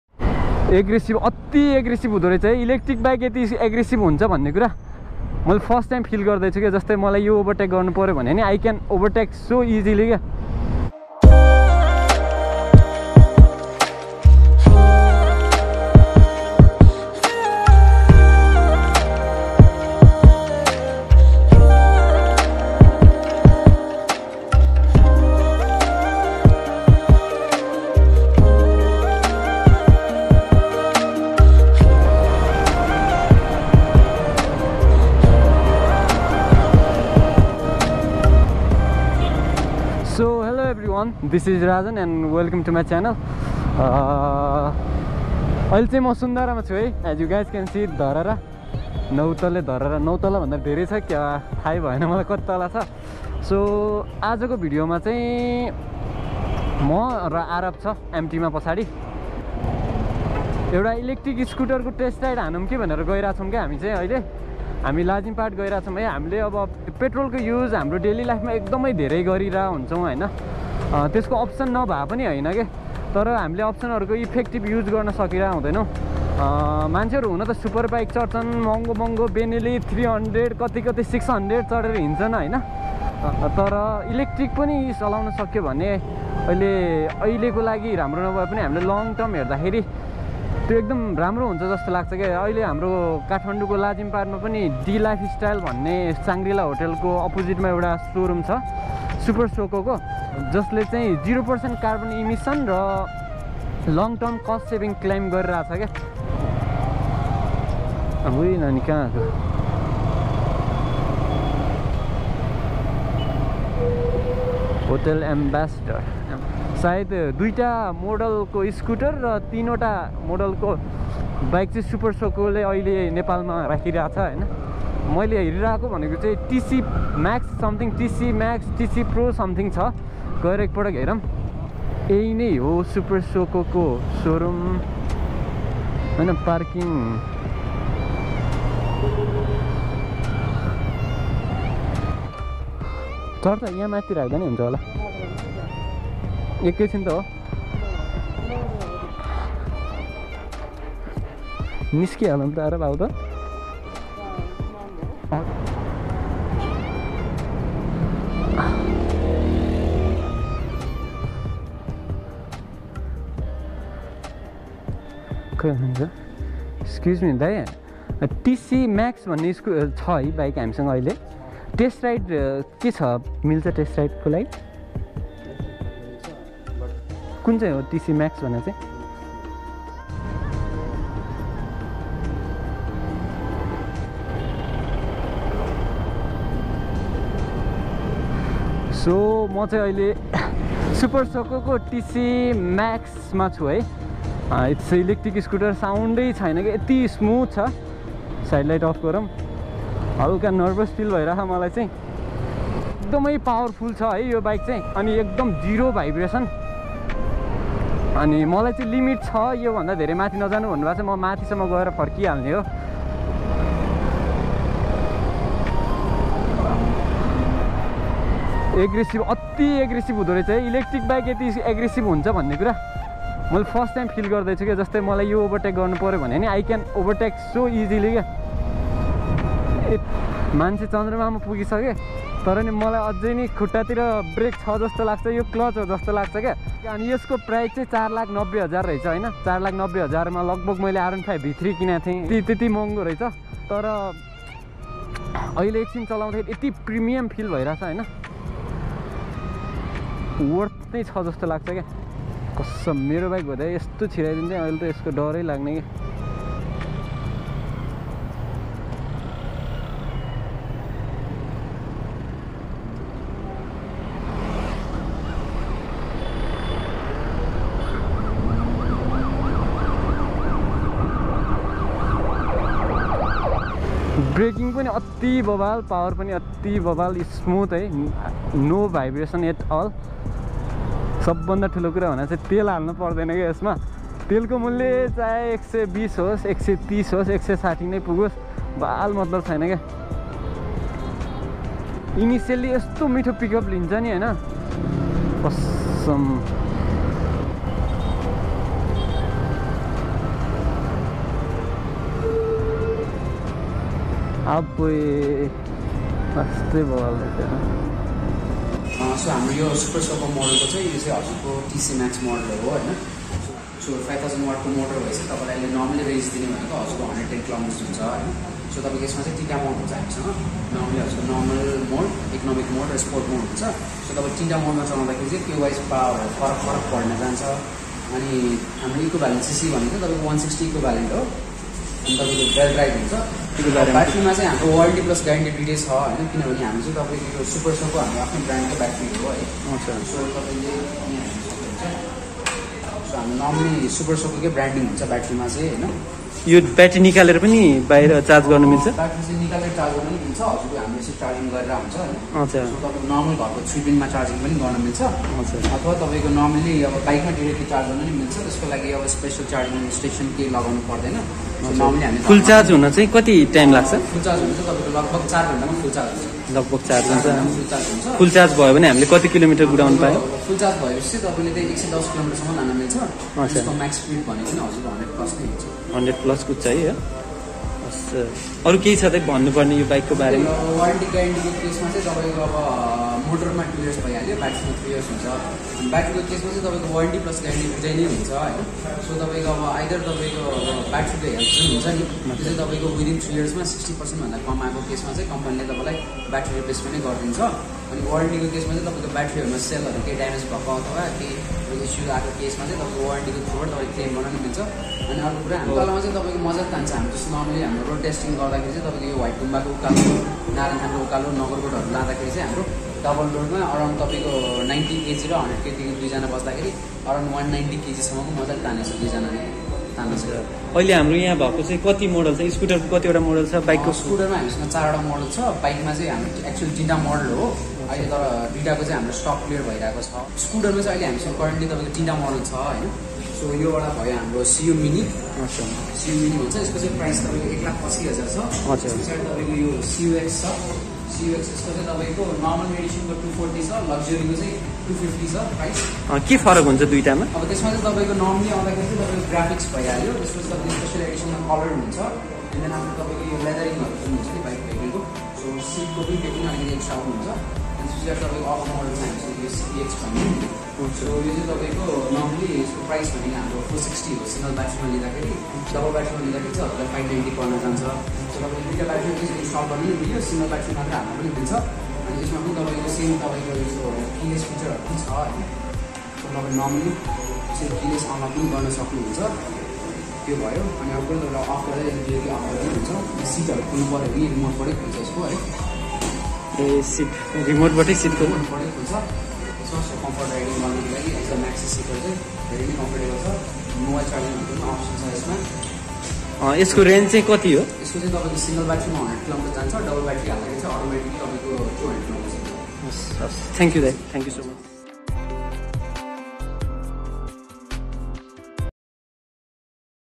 New Electric Bike in Nepal || Super Soco TC MAX Test Ride and Review || @imRaZan Vlogs
Reuploaded because of Quality issue 😁
Just trying to be Regular with some random vlogs.
Let's Promote electric vehicles in our country together.

For more details about Super Soco's electric bikes and scooters please contact Super Coco Nepal at Lazimpat, Kathmandu.